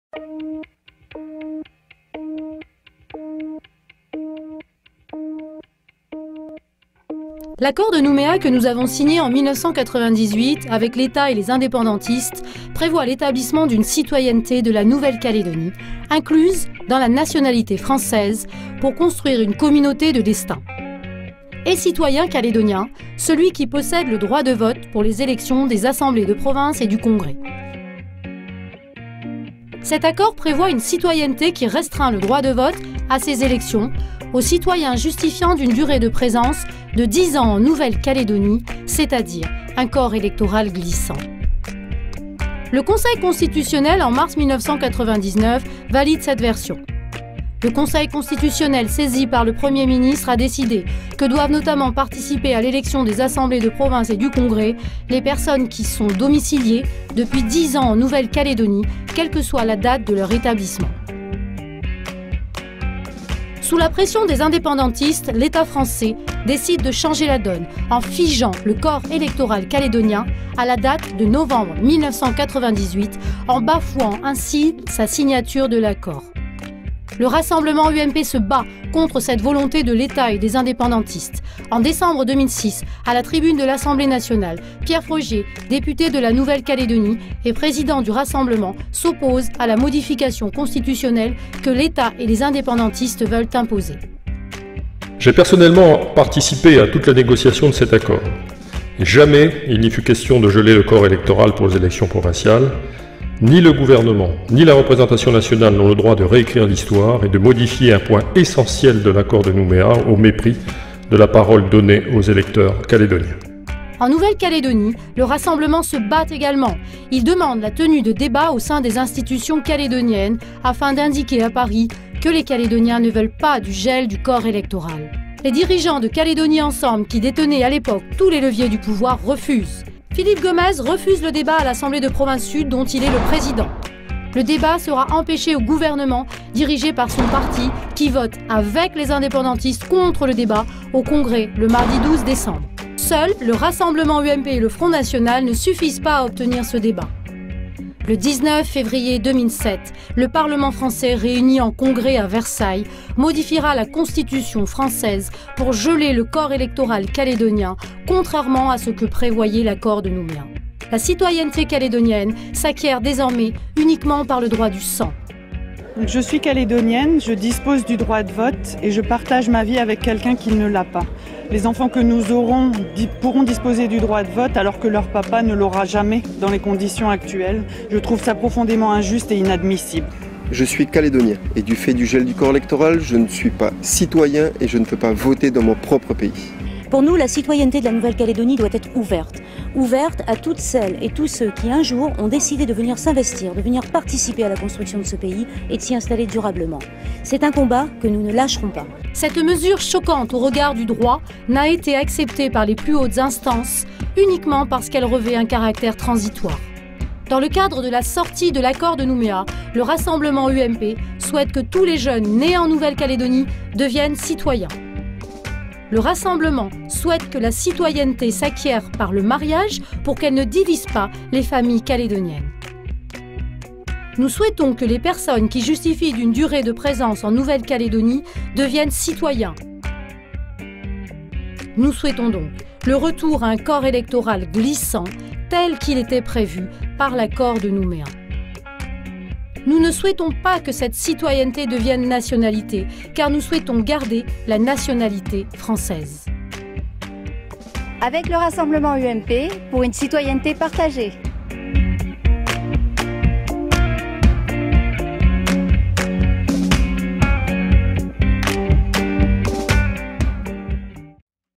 Pour une citoyenneté partagée - Par le Rassemblement-UMP
Parce que le Rassemblement-UMP est contre le gel du corps électoral.
Parce que le Rassemblement-UMP est pour une citoyenneté partagée.